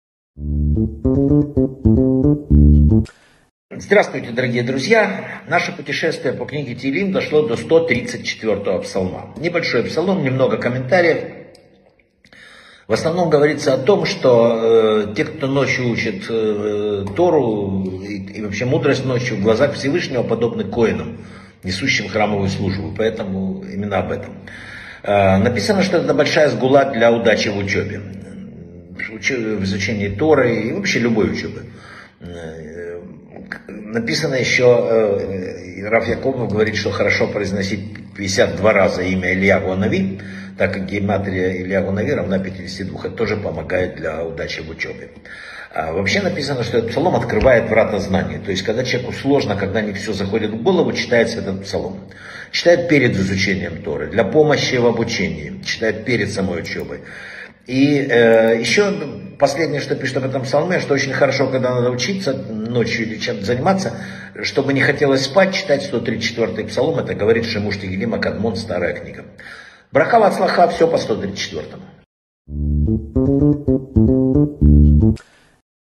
📗 Псалмы Давида. Псалом 134. Ценность ночных уроков Торы. Метод для успеха в учебе | Вадим Рабинович
С кем сравниваются люди, занимающиеся изучением Торы и познанием мудрости в ночное время суток?
Большая сгула для удачи в изучении Торы и любой учебе. Какое средство приводил рав Якобов?
Открытие врат знаний для человека. Чтение как перед непосредственно началом учебы, так и с просьбой к Вс-вышнему о помощи в изучении Торы.
Что говорит книга "Шимуш Теилим аКадмон" о том, какова польза этого Псалма в случаях, когда человеку необходимо учиться ночью?